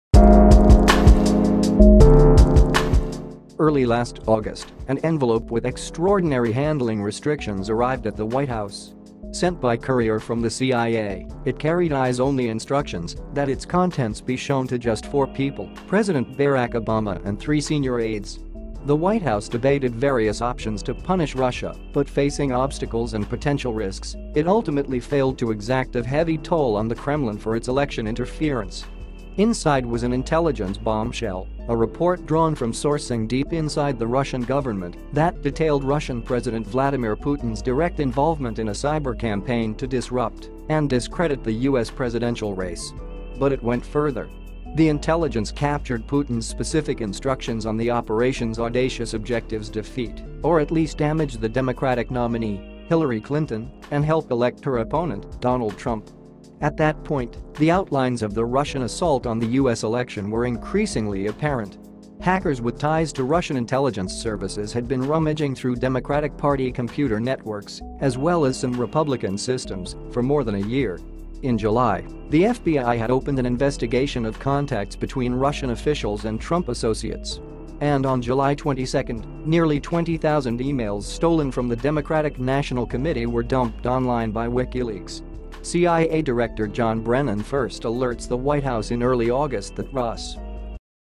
Obama’s secret struggle to punish Russia for Putin’s election assault
Early last August, an envelope with extraordinary handling restrictions arrived at the White House. Sent by courier from the CIA, it carried eyes only instructions that its contents be shown to just four people: President Barack Obama and three senior aides.

The White House debated various options to punish Russia, but facing obstacles and potential risks, it ultimately failed to exact a heavy toll on the Kremlin for its election interference.

Inside was an intelligence bombshell, a report drawn from sourcing deep inside the Russian government that detailed Russian President Vladimir Putins direct involvement in a cyber campaign to disrupt and discredit the U. S. presidential race.

But it went further. The intelligence captured Putins specific instructions on the operations audacious objectives  defeat or at least damage the Democratic nominee, Hillary Clinton, and help elect her opponent, Donald Trump.

At that point, the outlines of the Russian assault on the U. S. election were increasingly apparent. Hackers with ties to Russian intelligence services had been rummaging through Democratic Party computer networks, as well as some Republican systems, for more than a year. In July, the FBI had opened an investigation of contacts between Russian officials and Trump associates. And on July 22, nearly 20,000 emails stolen from the Democratic National Committee were dumped online by WikiLeaks.

CIA Director John Brennan first alerts the White House in early August that Russ